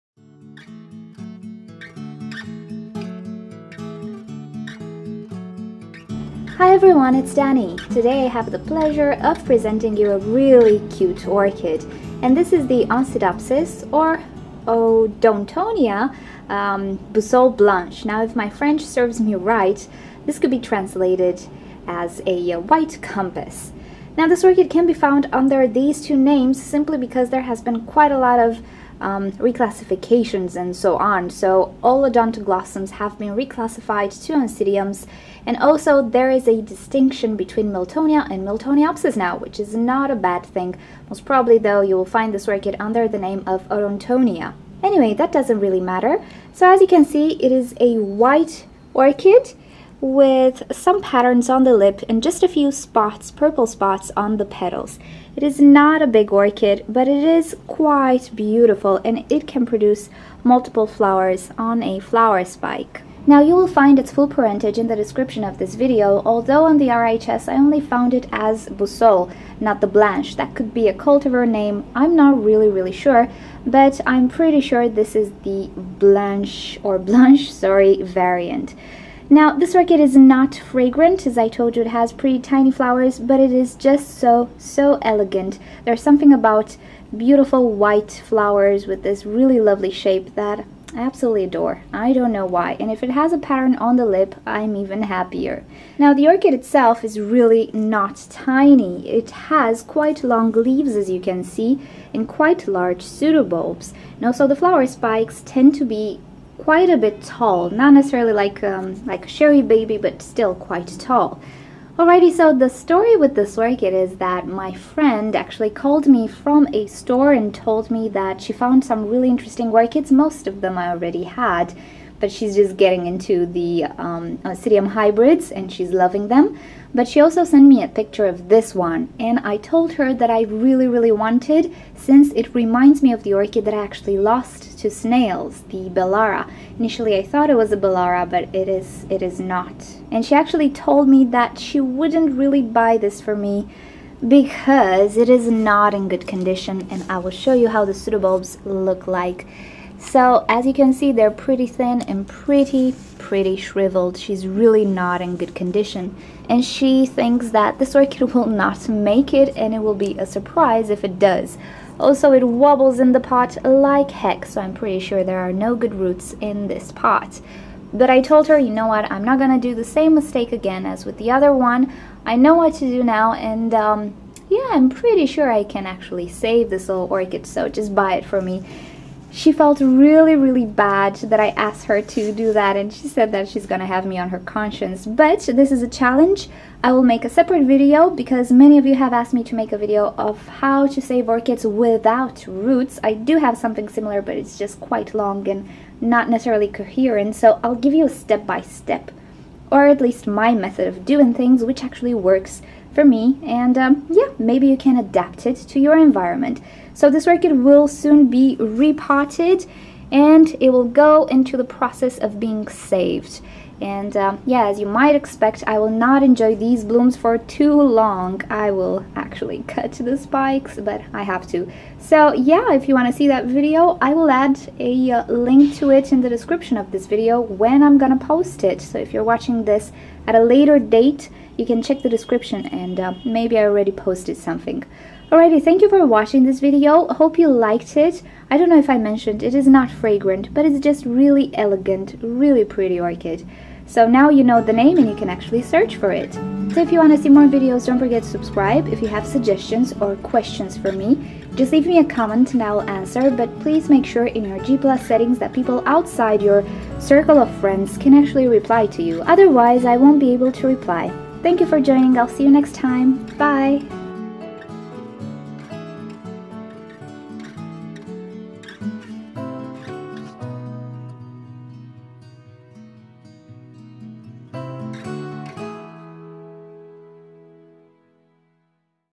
Oncidopsis (Odontonia) Boussole 'Blanche' - one very elegant white orchid
The Oncidopsis, or Odontonia, Boussole Blanche is a cross between Miltoniopsis Princess Mary and Oncidium Nabab. It has medium sized white flowers with dark red markings on the lip and few purple spots on the wihte petals, though not all flowers display them. It is not fragrant, but it is elegant :) Enjoy!